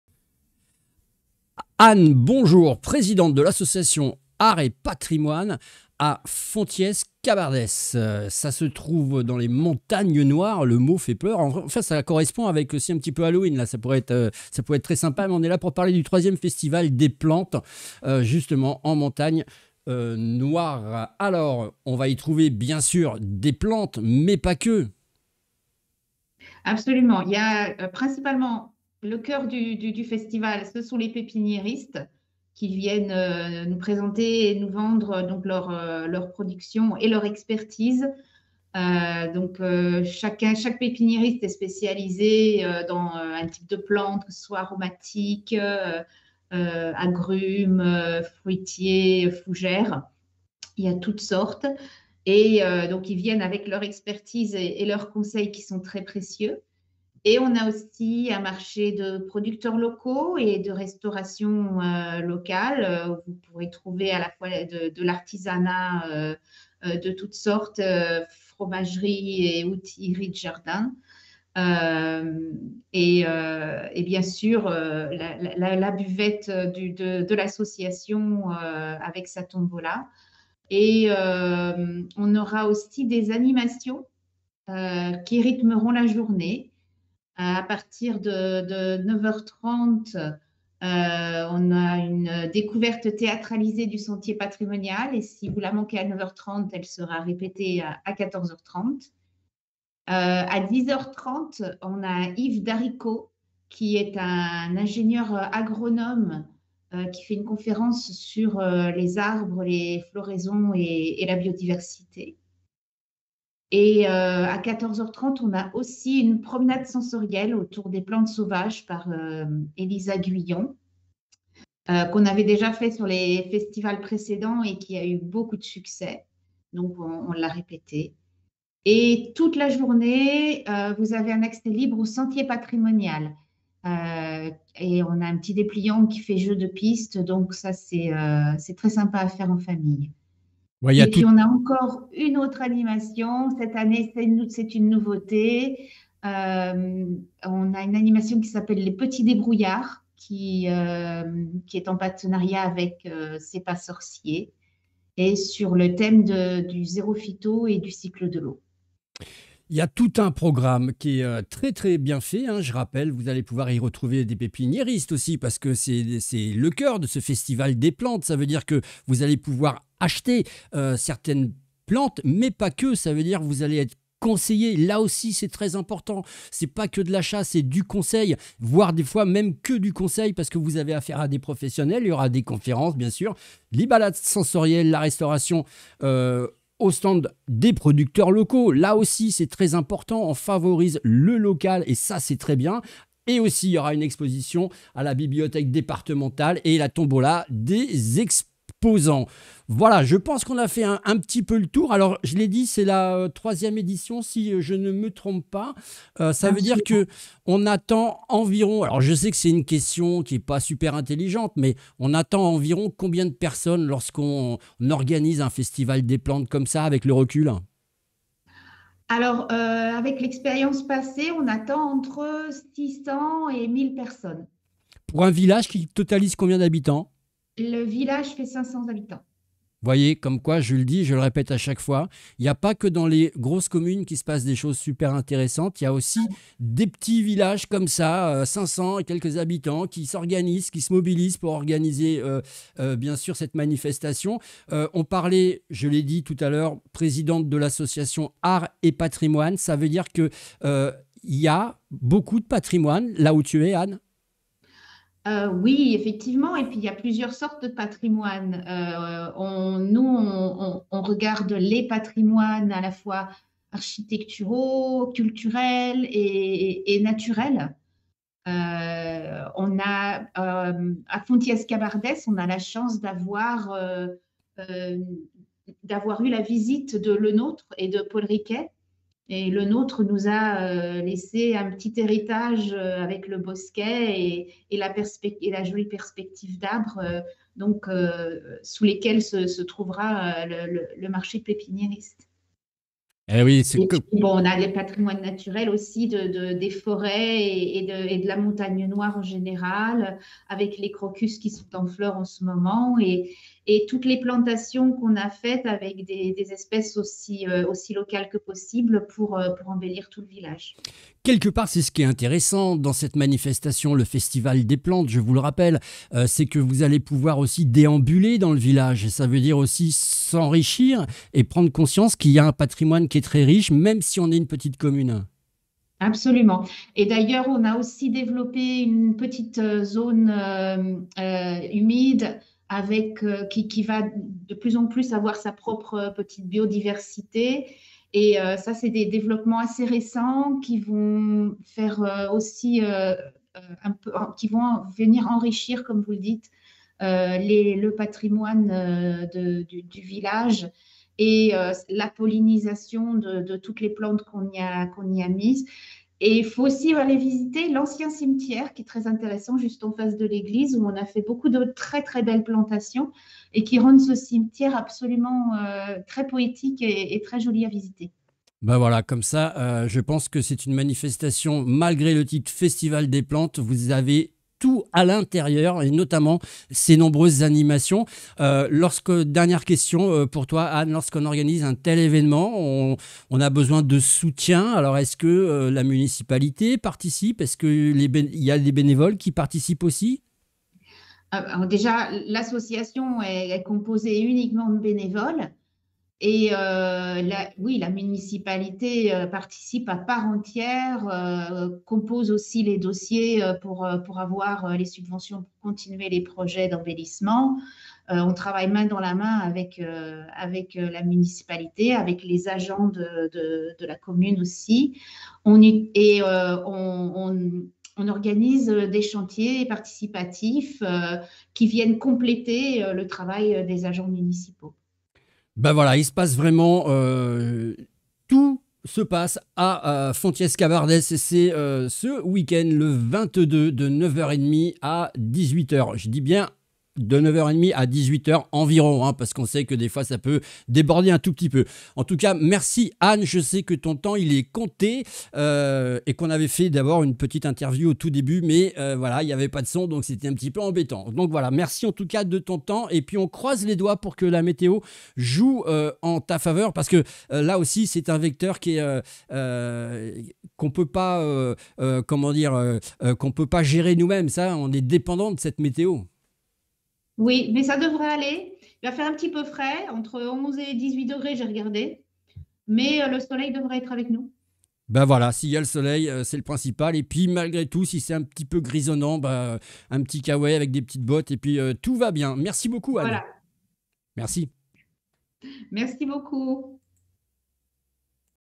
3ème festival des plantes en montagne noire, Fontiers-Cabardès (Aude) le 22/10 de 10 à 18 h
Interview Anne Lesné, Présidente de l’association Arts et Patrimoines de Fontiers-Cabardès